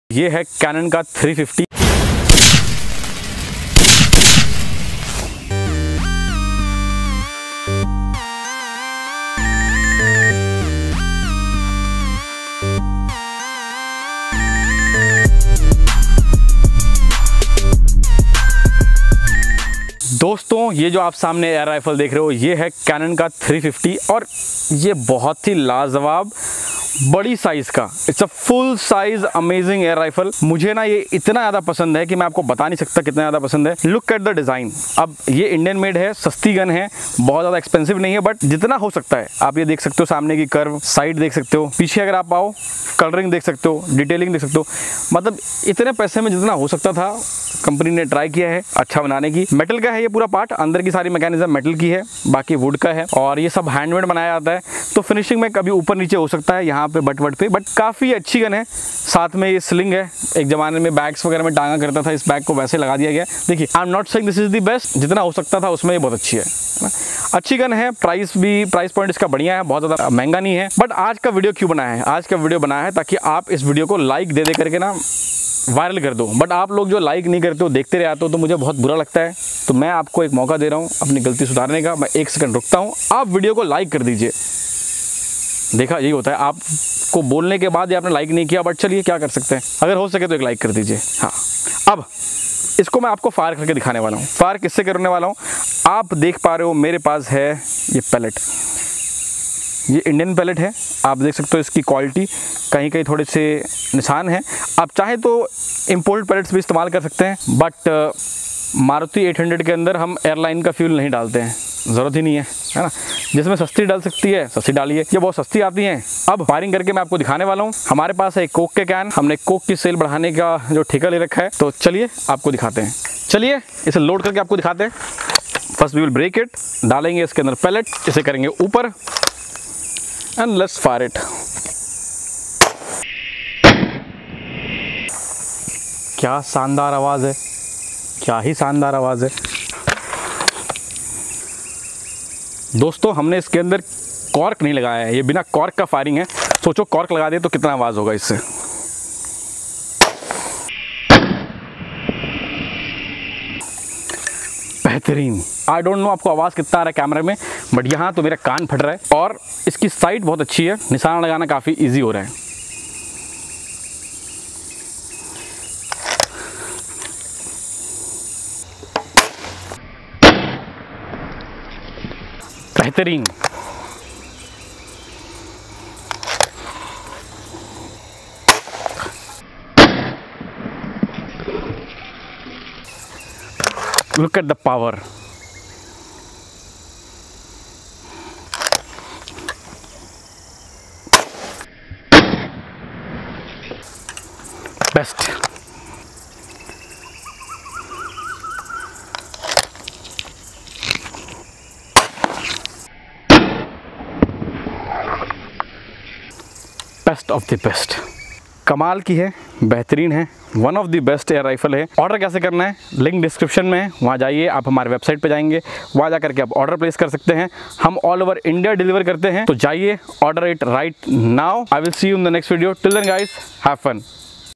Canon 350 vs Coke Can shooting test
If you're someone who enjoys shooting and is looking for an affordable air rifle, then the Canon 350 Sports Air Rifle by Airsoft Gun India is definitely worth considering. This Indian-made air rifle is both accurate and powerful, making it perfect for target practice

In this video, we'll be testing the Canon 350 Sports Air Rifle against a Coke can. We'll be shooting at a distance of 10 meters to see just how accurate and powerful this air rifle really is. With its smooth trigger and scope rail, we're confident that it will perform well.

The Canon 350 Sports Air Rifle is made of metal and wood and has a weight of 3.6 kg. It uses .177 Indian pellets as ammo and has a range of 150 fps. The target range is between 10-20 meters, making it perfect for backyard shooting. It's important to note that no license is required to purchase this air rifle, but you will need to provide proof of identification such as your Aadhar, driving license, voter ID, or passport.

While this air rifle is partially handmade and partially machine-made, it may not have a lavish finish, and there may be scratches on the barrel or stock. It's also important to note that it's a single-shot air rifle, so you'll need to reload after each shot.

If you're interested in purchasing the Canon 350 Sports Air Rifle, you can order it through the Airsoft Gun India website or by contacting them via WhatsApp or phone call. It's an affordable air rifle that's perfect for beginners or those on a budget.

Overall, the Canon 350 Sports Air Rifle is a great option for those looking for an affordable air rifle that's both accurate and powerful. Be sure to check out our video to see just how well it performs against a Coke can, and don't forget to like and subscribe for more air rifle reviews and tests.